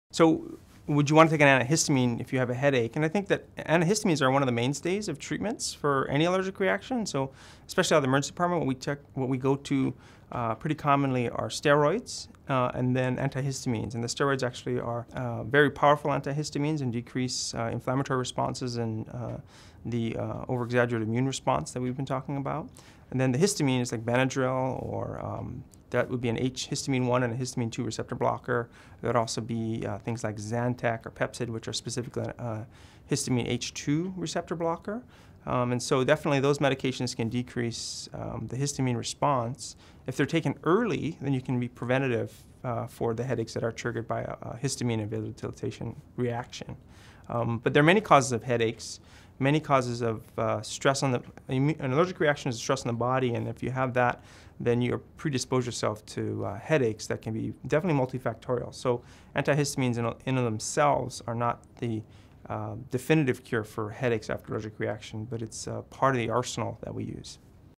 Do Antihistamines Help Headaches After Allergic Reactions? - David Feldman, MD - Emergency Medicine
Antihistamines are one of the main drugs used to treat allergic reactions. In this video, emergency medicine physician, David Feldman, MD, of Good Samaritan Hospital explains whether they're effective in treating headaches, too. For more information please visit our